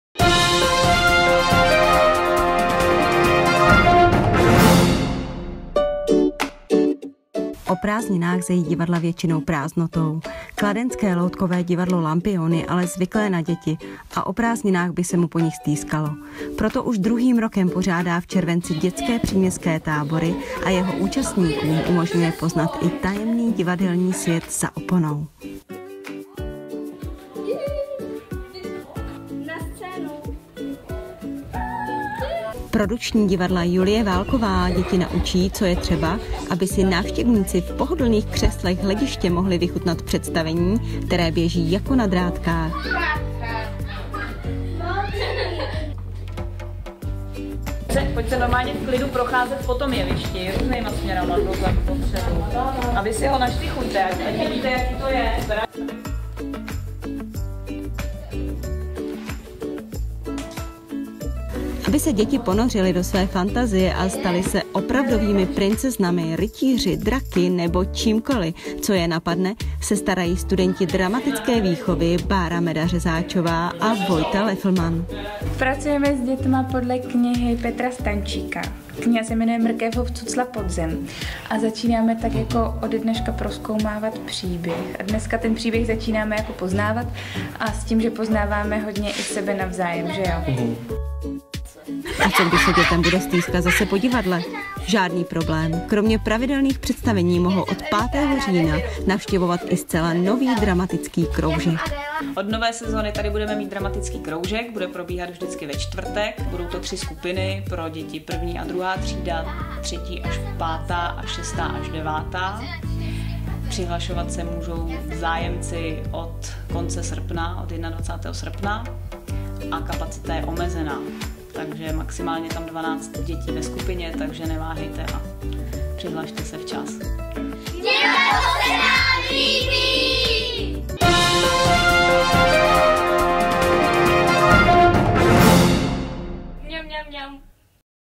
Příměstský dětstký tábor v Divadle Lampion
Loutkové Divadlo Lampion pořádá již druhým rokem příměstské letní tábory. Děti tak mají možnost poznat i tajemný svět za divadelní oponou.